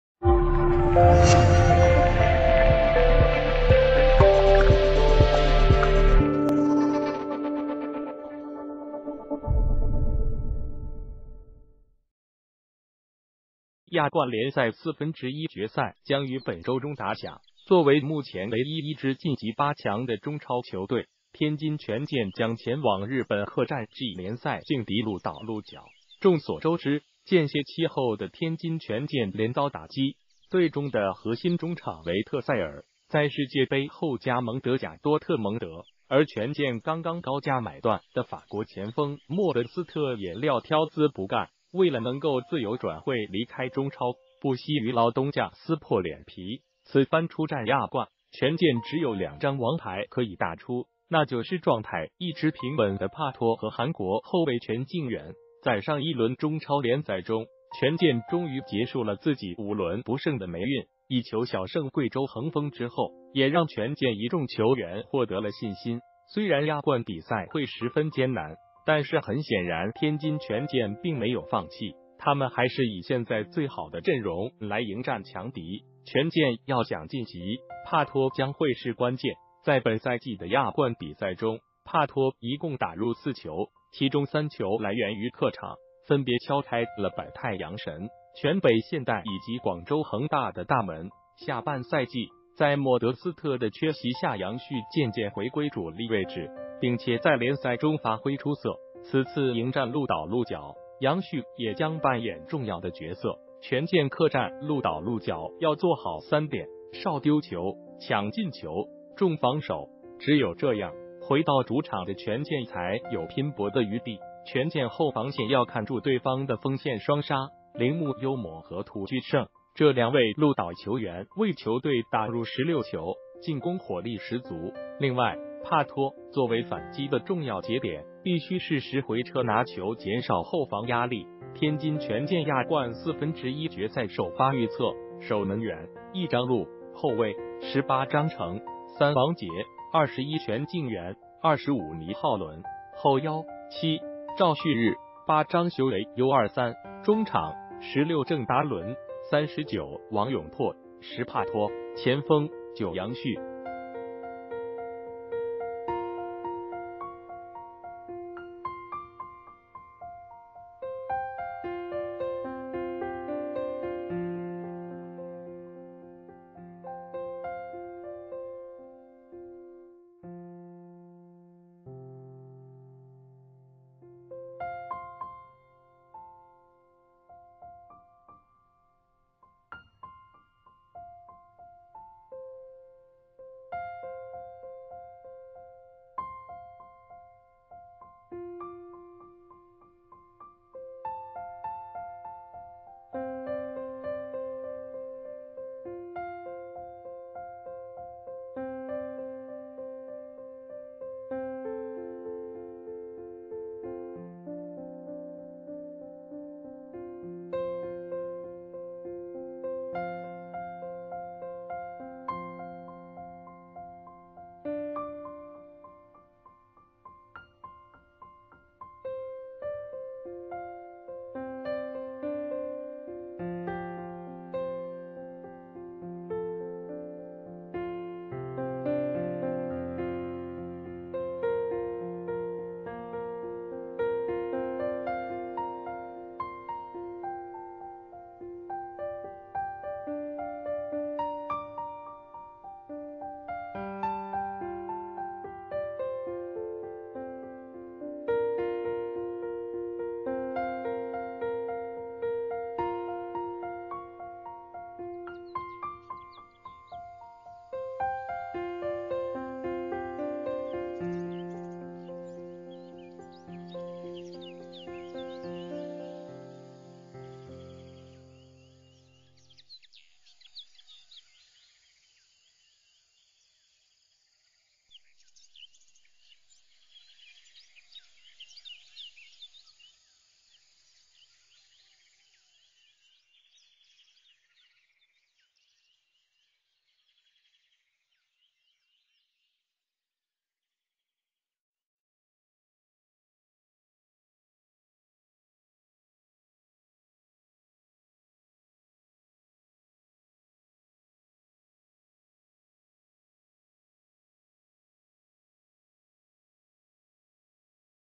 天津权健亚冠1/4决赛首发预测：帕托领衔出战，杨旭张修维首发！
谢谢！亚冠联赛1/4决赛将于本周中打响。作为目前唯一一支晋级8强的中超球队，天津权健将前往日本客战J联赛劲敌鹿岛鹿角。众所周知，间歇期后的天津权健连遭打击，队中的核心中场维特塞尔在世界杯后加盟德甲多特蒙德。而权健刚刚高价买断的法国前锋莫德斯特也撂挑子不干，为了能够自由转会离开中超，不惜与老东家撕破脸皮！此番出战亚冠，权健只有两张王牌可以打出，那就是状态一直平稳的帕托和韩国后卫权敬源。在上一轮中超联赛中，权健终于结束了自己5轮不胜的霉运，1球小胜贵州恒丰之后，也让权健一众球员获得了信心。虽然亚冠比赛会十分艰难，但是很显然天津权健并没有放弃！他们还是以现在最好的阵容来迎战强敌。权健要想晋级，帕托将会是关键！在本赛季的亚冠比赛中，帕托一共打入4球。其中3球来源于客场，分别敲开了柏太阳神，全北现代以及广州恒大的大门。下半赛季，在莫德斯特的缺席下杨旭渐渐回归主力位置，并且在联赛中发挥出色，此次迎战鹿岛鹿角，杨旭也将扮演重要的角色。权健客战鹿岛鹿角要做好3点：少丢球，抢进球，重防守！只有这样，回到主场的权健才有拼搏的余地。权健后防线要看住对方的锋线双煞：铃木优磨和土居圣。这两位鹿岛球员为球队打入16球，进攻火力十足！另外，帕托作为反击的重要节点必须适时回撤拿球减少后防压力。天津权健亚冠1/4决赛首发预测：守门员：1-张鹭；后卫：18-张诚、3-王杰、21-权敬源、25-糜昊伦；后腰：7-赵旭日、8-张修维（U23）；中场：16-郑达伦、39-王永珀、10-帕托；前锋：9-杨旭